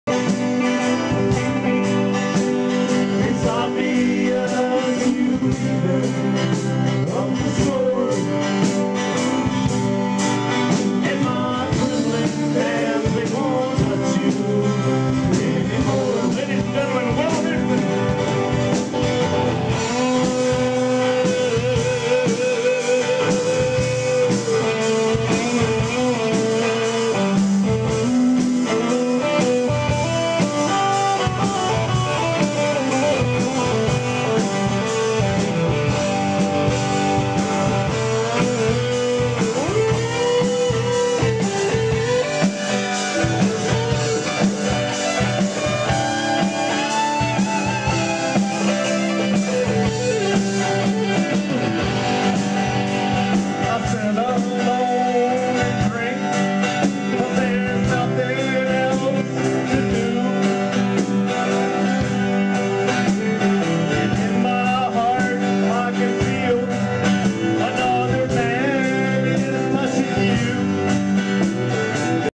Charles Cullen 3D Video Anaglyph
Charles singing at the Bamboo Bingola March 7, 2010